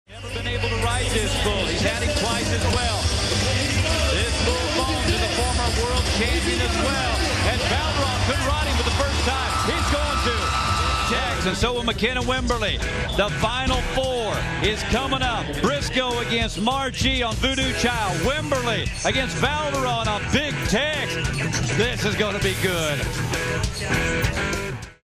Valdiron de Oliveira Vs Bones
de Oliveira rides 2008 World Champ bull Bones on way to Iron Cowboy victory at Cowboys Stadium.